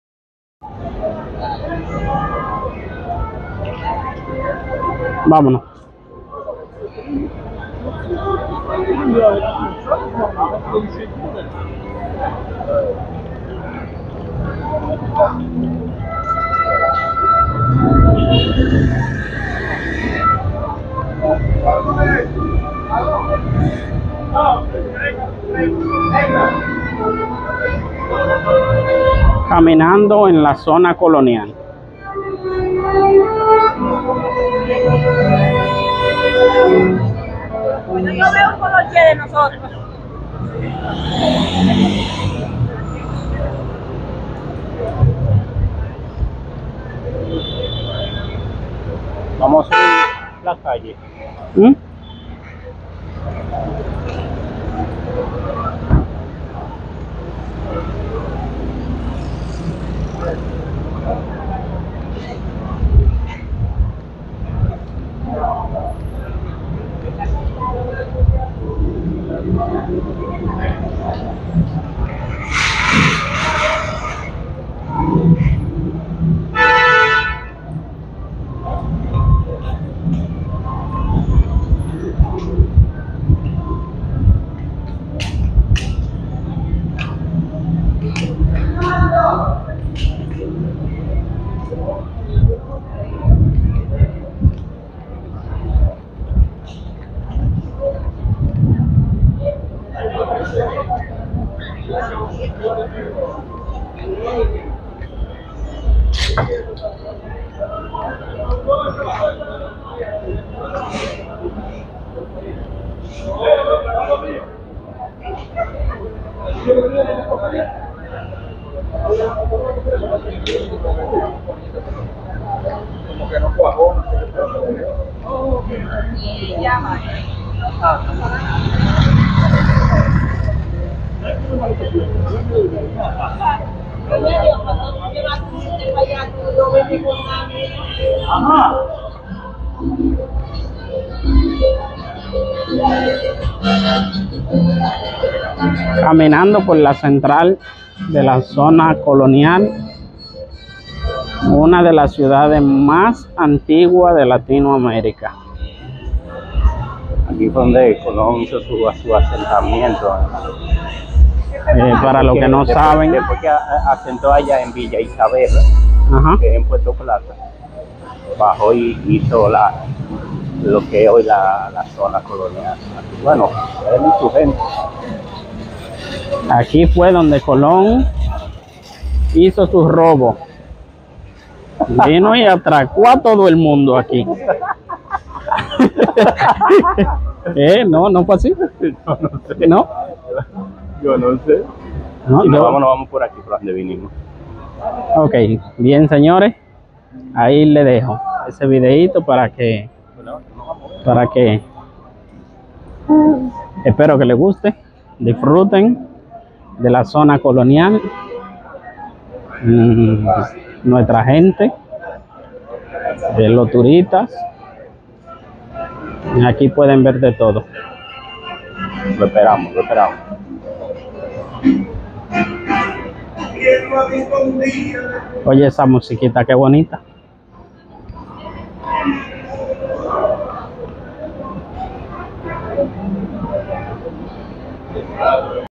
Explorando la Riqueza Histórica: Un Paseo por la Zona Colonial de Santo Domingo.
En este video, te invitamos a un fascinante recorrido por la Zona Colonial de Santo Domingo, un lugar que guarda siglos de historia y encanto en cada una de sus calles empedradas. Sumérgete en un ambiente lleno de arquitectura colonial, estrechas callejuelas y monumentos impresionantes mientras caminamos por este emblemático sitio. Descubre la magia de sus plazas, iglesias y fortalezas que nos transportan a tiempos pasados.

Algo bueno Pronto.